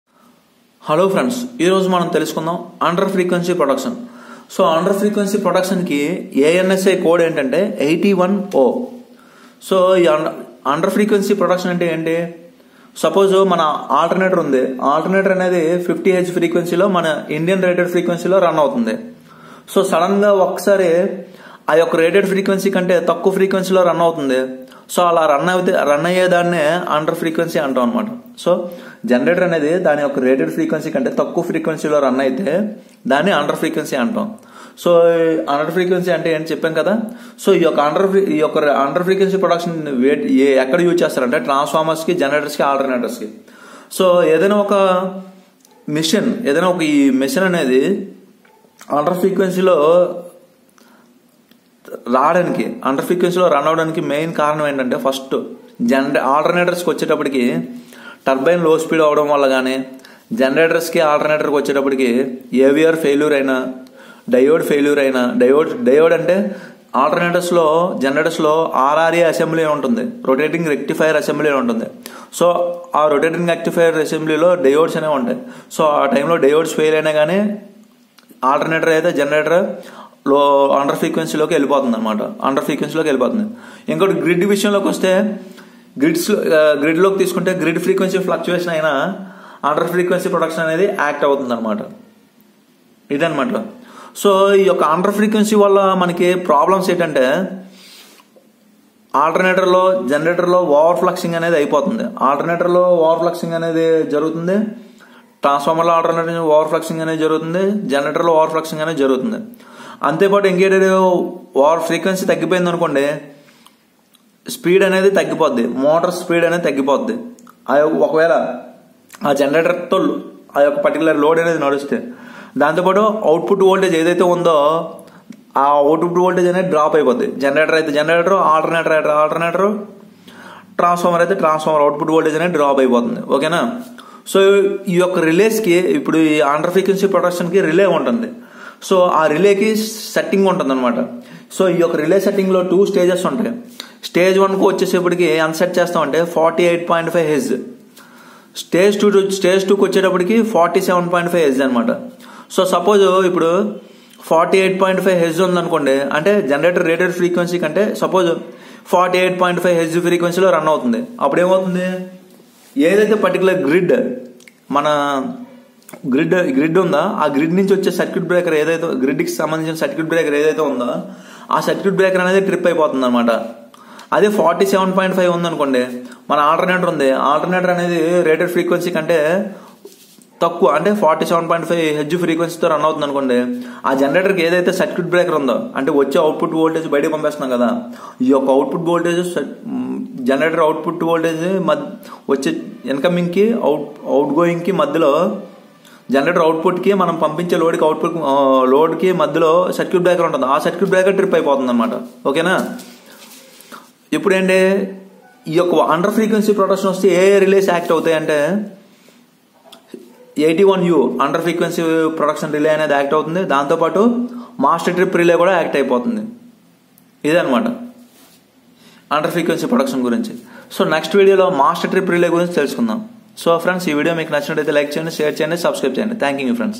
Under frequency protection relay for alternator generator transformer in Telugu 2019
Hi friends in this video u will learn about Under frequency protection relay for alternator generator transformer in Telugu 2019